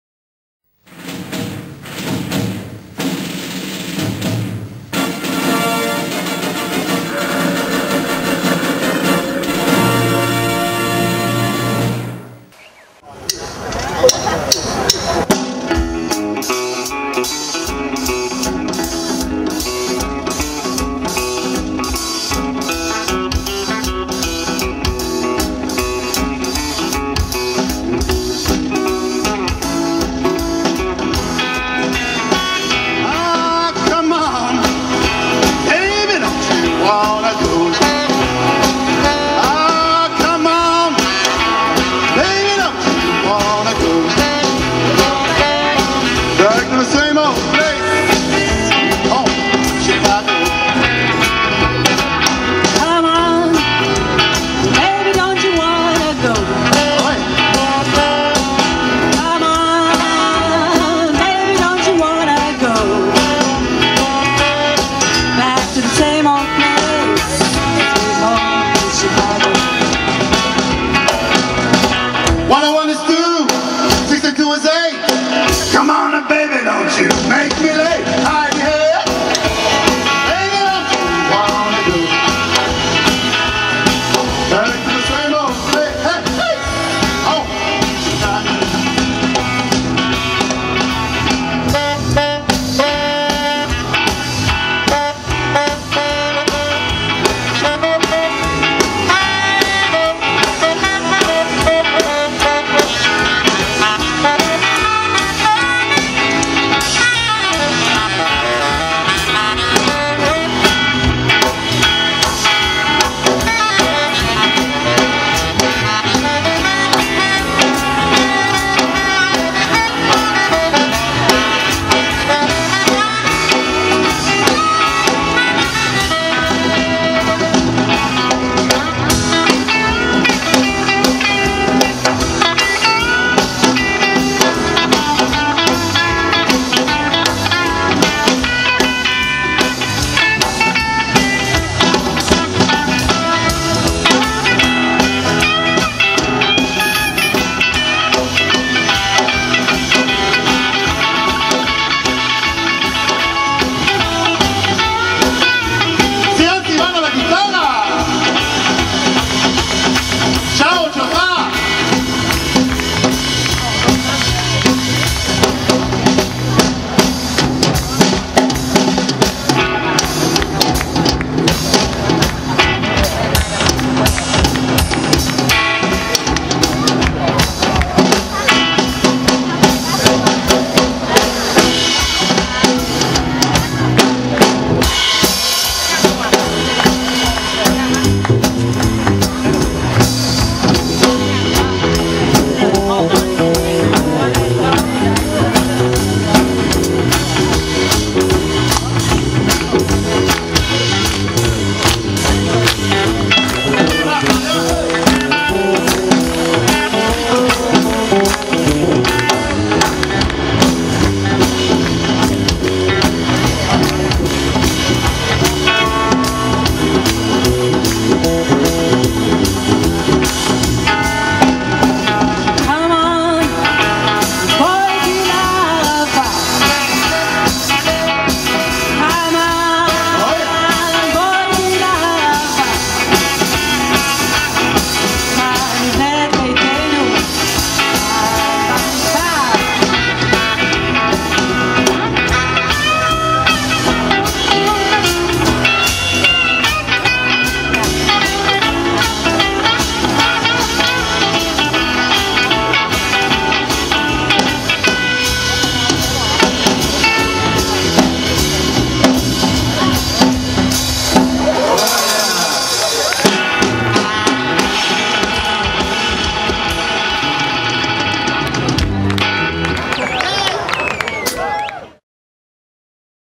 Desert Cooler - Arava - Israel
מוגש לפניכם קליפ של הרכב החקלאים מהערבה Desert Cooler
שאול שובל - תופים, אייל פוליקר - גיטרה ושירה, ציון סיוון - גיטרה חשמלית, מעיין קיטרון - שירה.
בועז הורויץ - סקסופון, זוהר ניר - קלידים.
אורחים נכבדים - שאול מוכתר  -סקסופון , יורם גולדהאמר - קלידים.
צילום ועריכה - אביבה פז ופני בן דוד.
The Desert Cooler makes a lot of noise but not very effective...